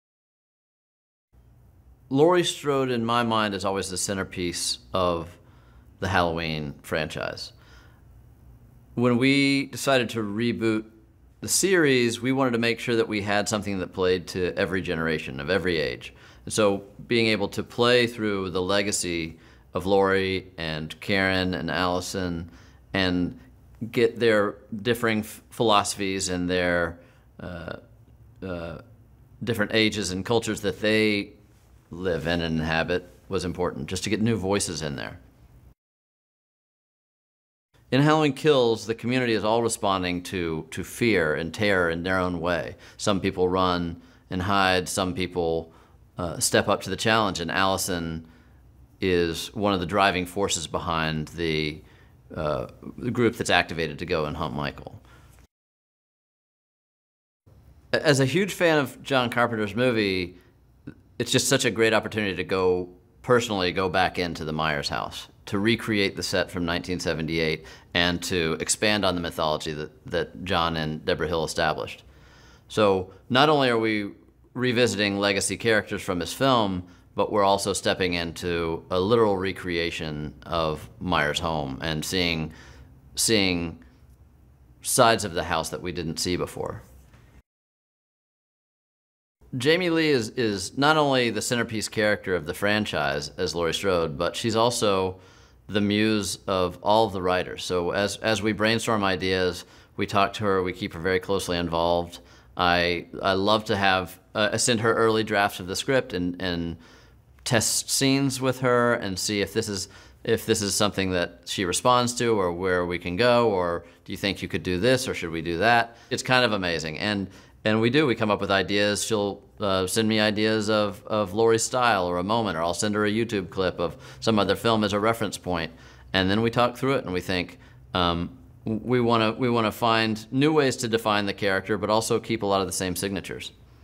Halloween Kills Soundbites David Gordon Green
DAVID GORDON GREEN/ Director - Co-Writer
Focusing on Laurie Strode and her family
The town's response to fear
Going back to the Myers House
Working with Jamie Lee Curtis

Visit us on the YouTube + Twitter:

Twitter • @ChicagoSciFi